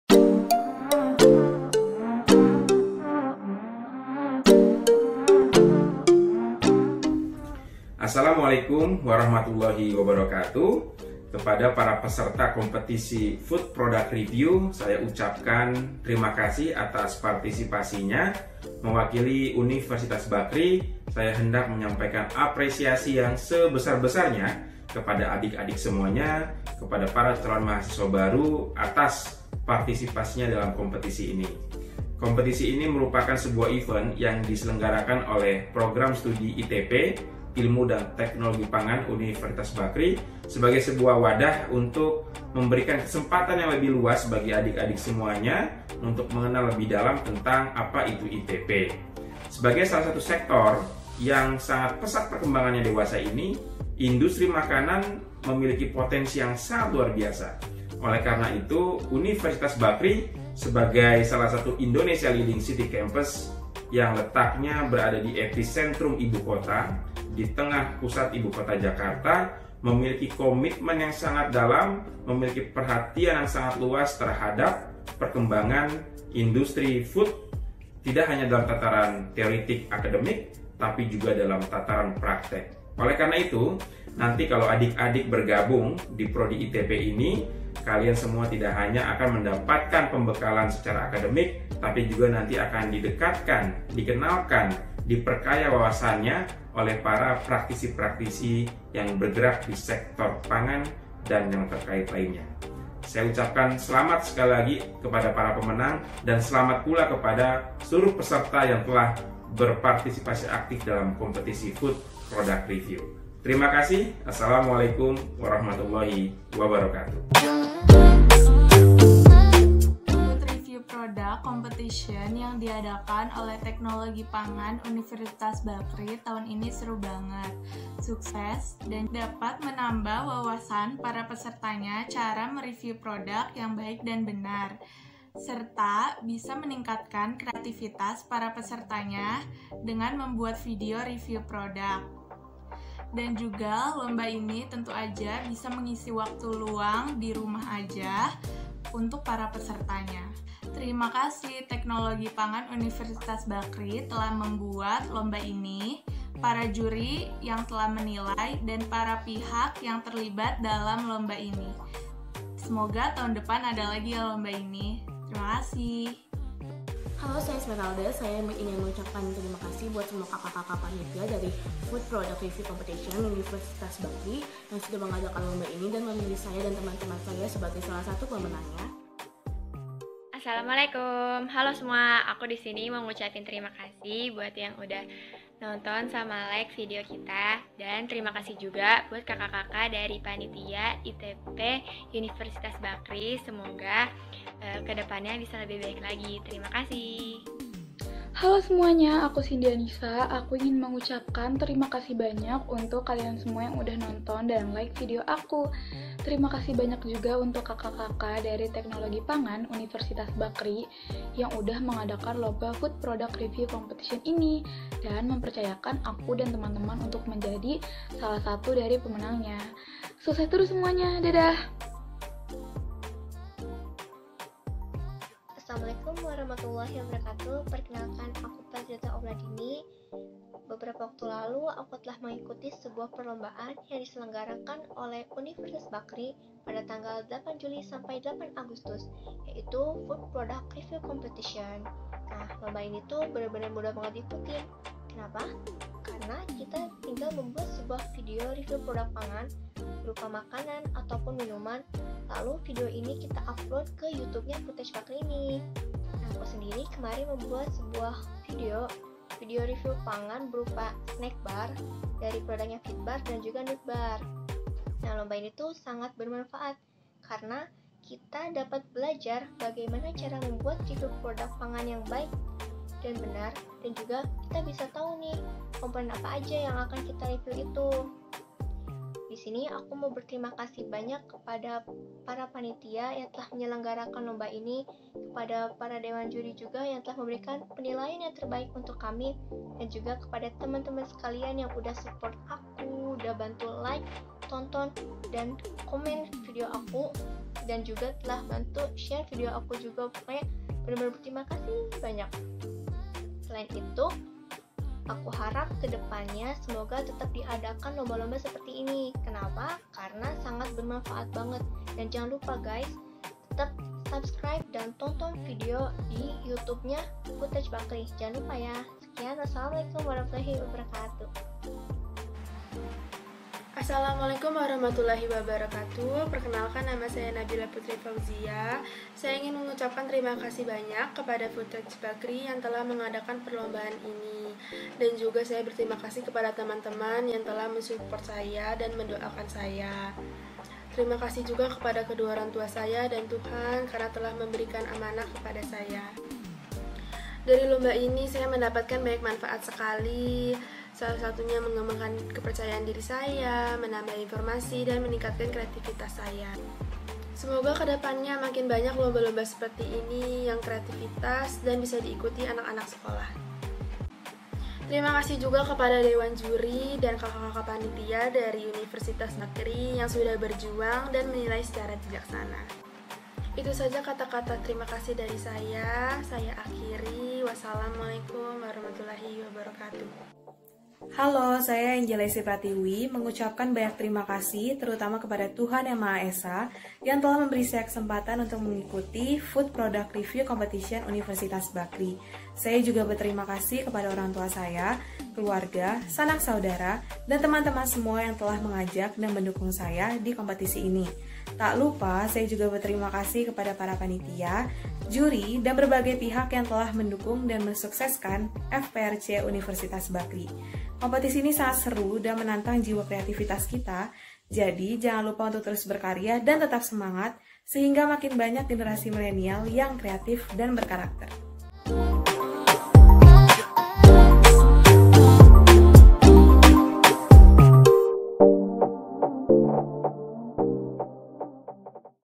Apa Kata Mereka tentang Food Product Review Competition?
Video ini berisi testimoni para pemenang Food Product Review Competition yang berlangsung pada 8 Juli hingga 8 Agustus 2020.
Video ini diawali oleh sambutan dari Bapak Wakil Rektor Universitas Bakrie mengenai ajang kompetisi ini.